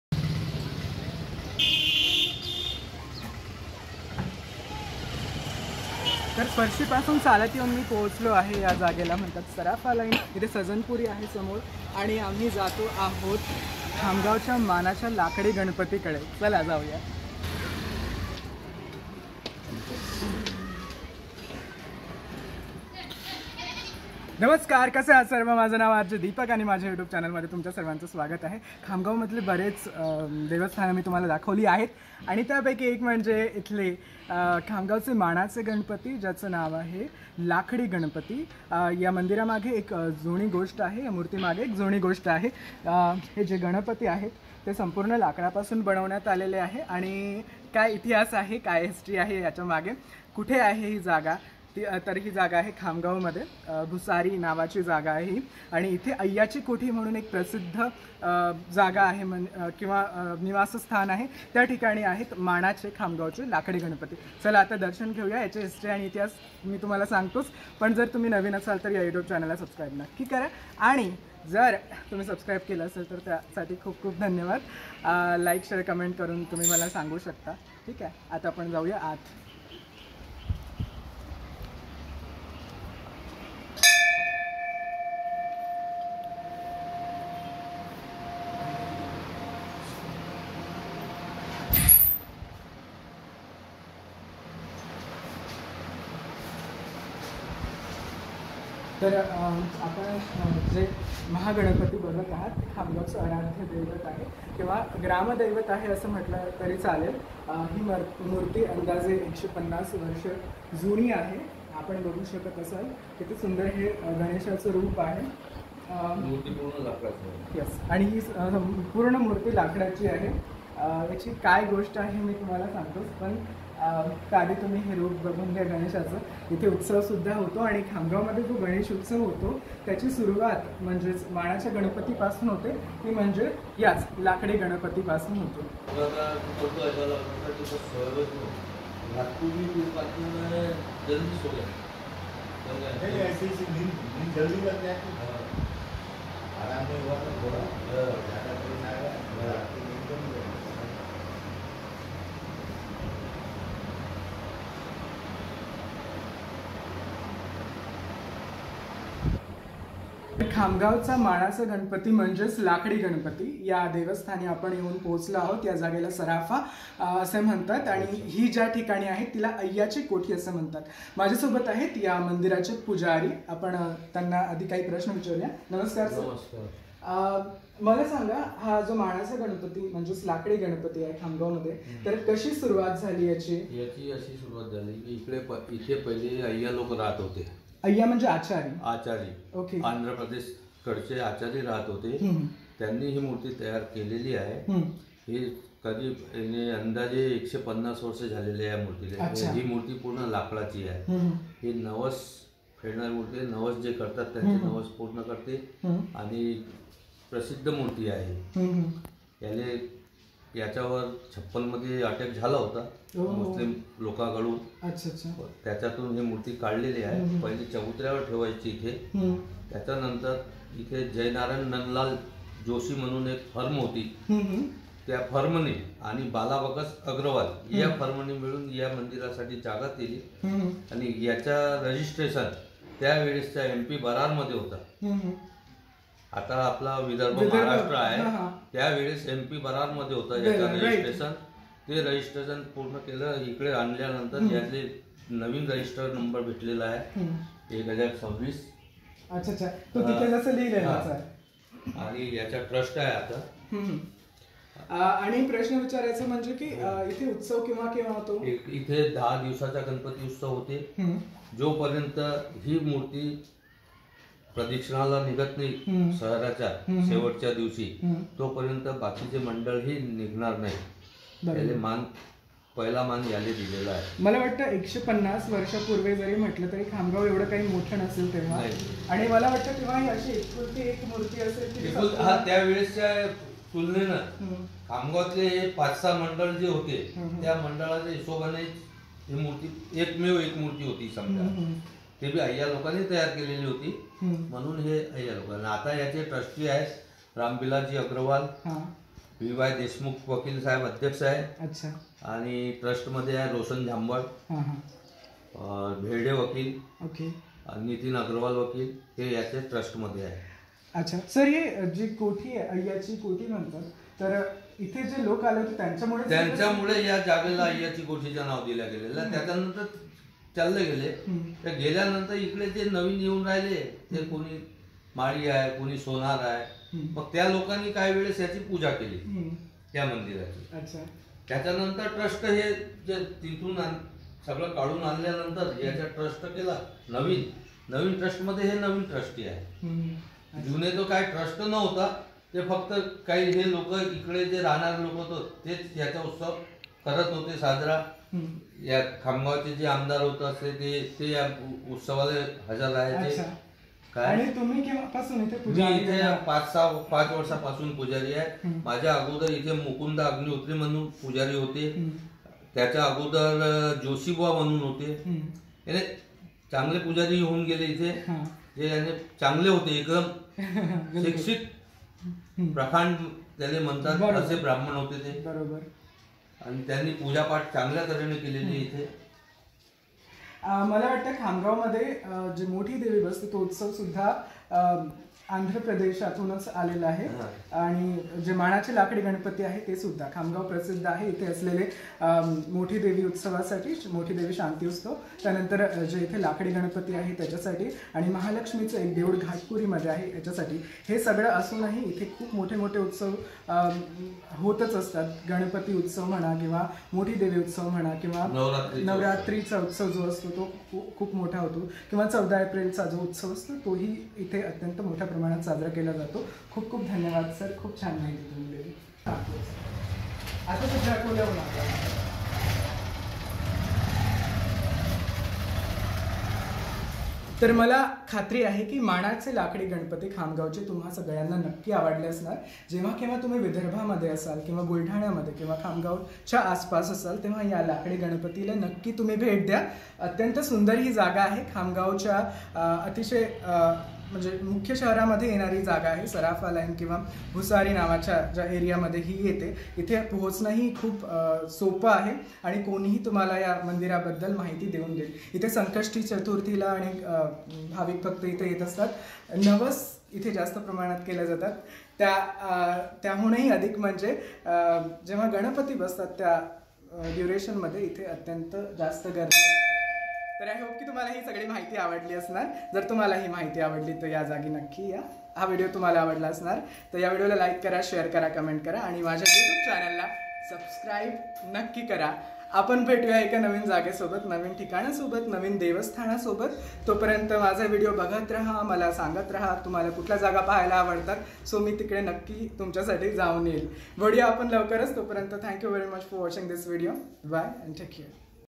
Shri Lakadi Ganpati | लाकडी गणपती | अय्याची कोठी | Khamgaon | Buldhana | Vidarbha Tourism | RJ Dipak
नमस्कार, आज बघा विदर्भातील बुलढाणा जिल्ह्यातील खामगांव शहरातील मानाचे श्री लाकडी गणपती, काय आहे इतिहास माहिती आणि दर्शनासाठी हा व्हिडीओ पुर्ण नक्की बघा लाईक कंमेंट शेअर तसेच नवनवीन व्हिडीओ पाहण्यासाठी या युट्यूब चॅनल ला सबस्क्राईब करायला विसरू नका..